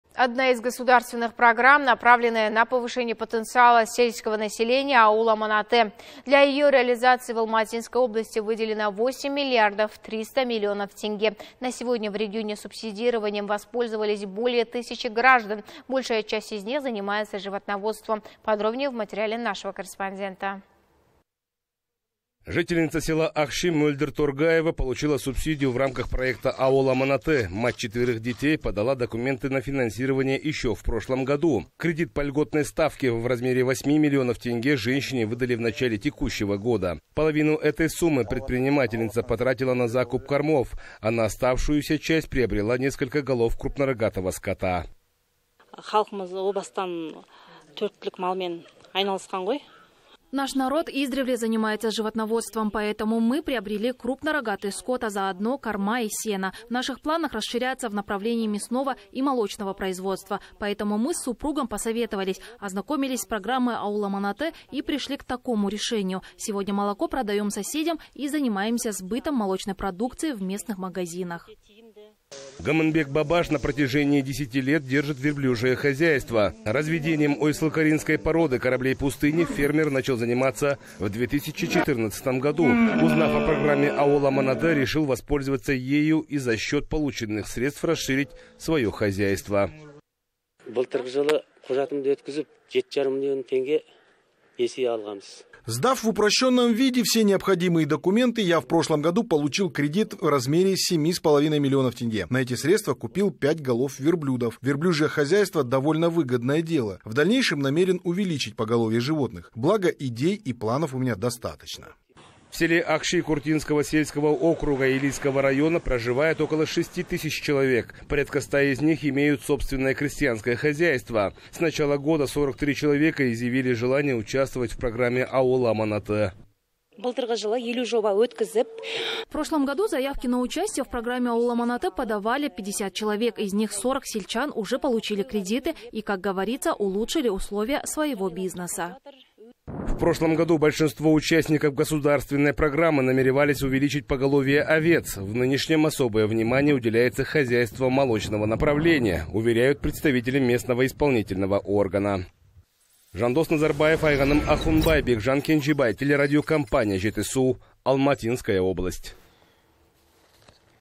Программа направленная на повышение потенциала сельского населения - «Ауыл аманаты»
Одна из государственных программ, направленная на повышение потенциала сельского населения, - «Ауыл аманаты». Для ее реализации в Алматинской области выделено 8 миллиардов 300 миллионов тенге. На сегодня, в регионе, субсидированием воспользовались более тысячи граждан. Большая часть из них занимается животноводством.